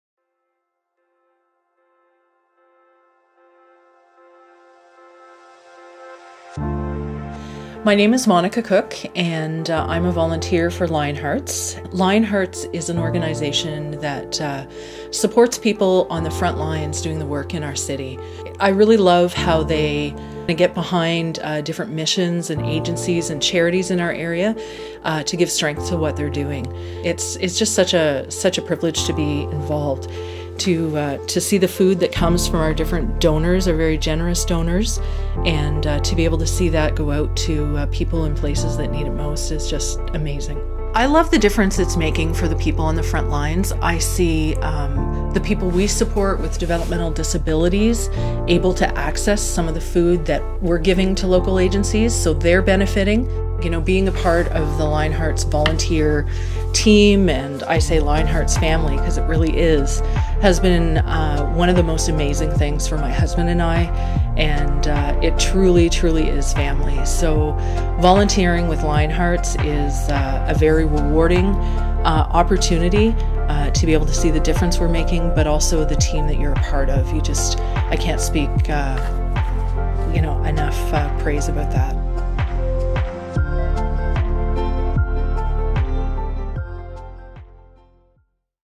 Lion Hearts Inc - Monica - 4K
Before COVID changed our day to day we had the blessing of working with an amazing organization from Kingston ON. Throughout this crisis, they have partnered with true hero’s of their community to bring meals by the thousands to the needy in the city. Here is a shout out to @lionhearts.inc If you would like to learn more check out their page and give them a follow. This is a short snippet of what we did.